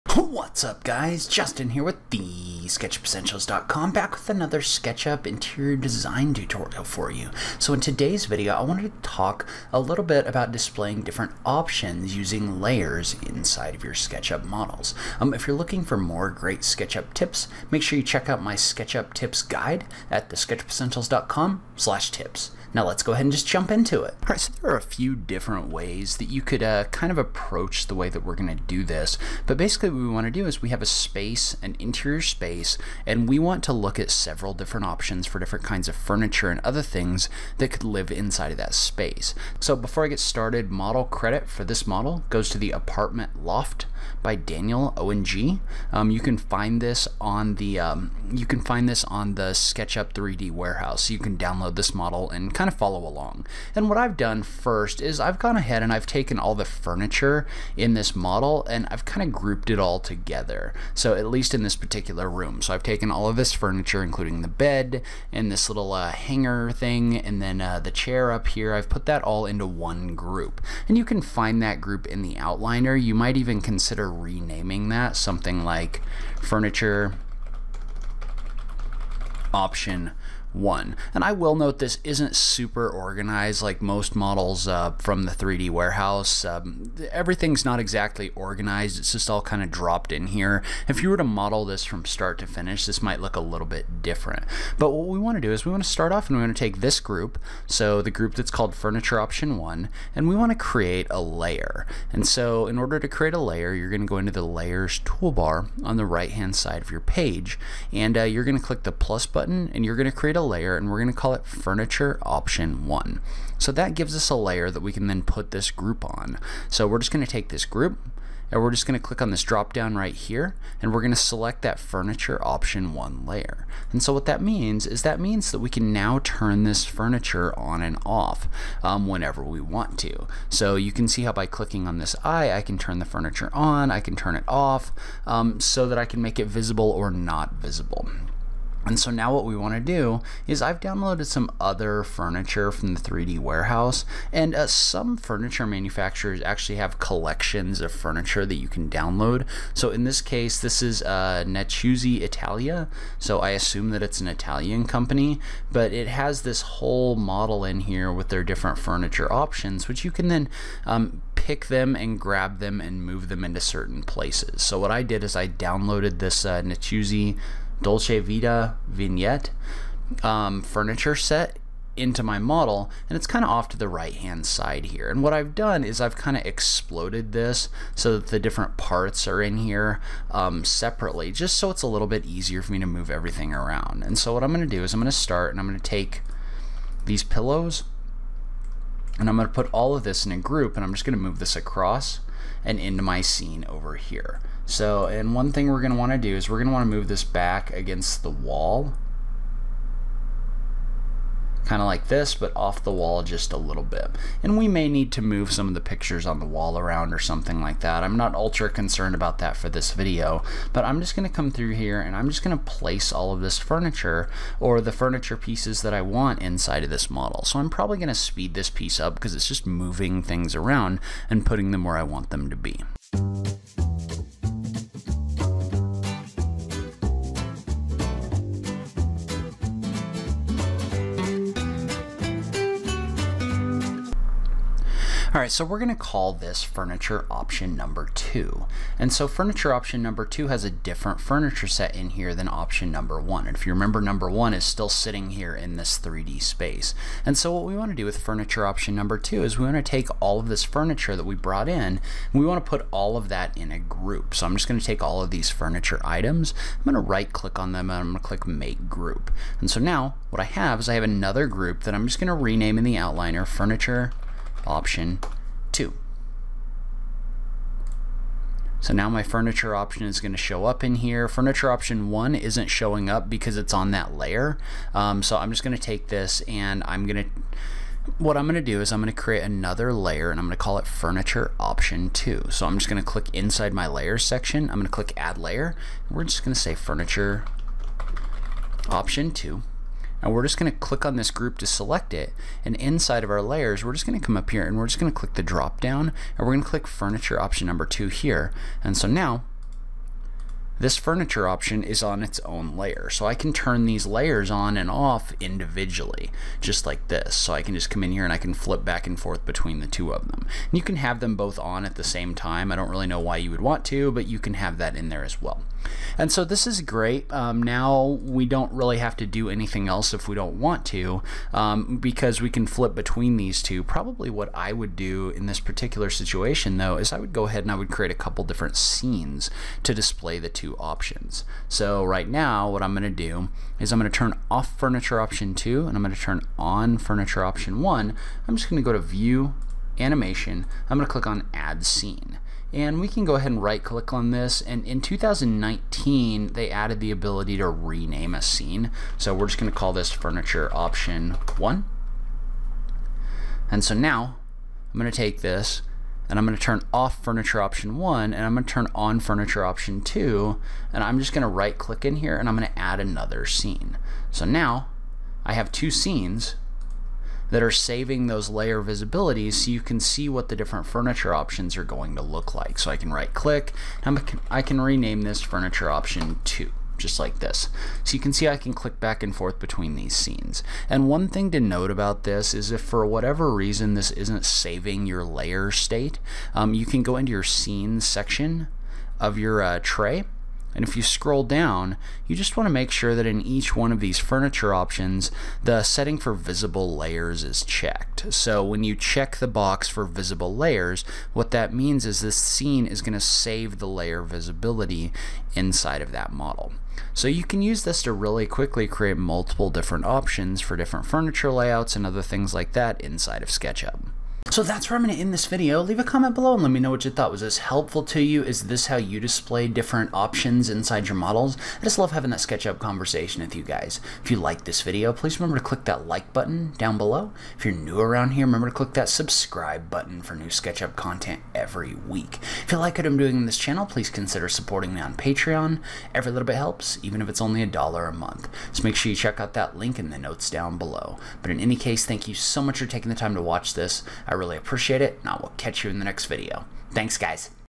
Showing Multiple Furniture Options in SketchUp - INTERIOR DESIGN TUTORIAL
In today's video, learn to organize your models so you can show off multiple different building or furniture configurations inside of SketchUp. This video can be be especially helpful for Interior Designers and Architects!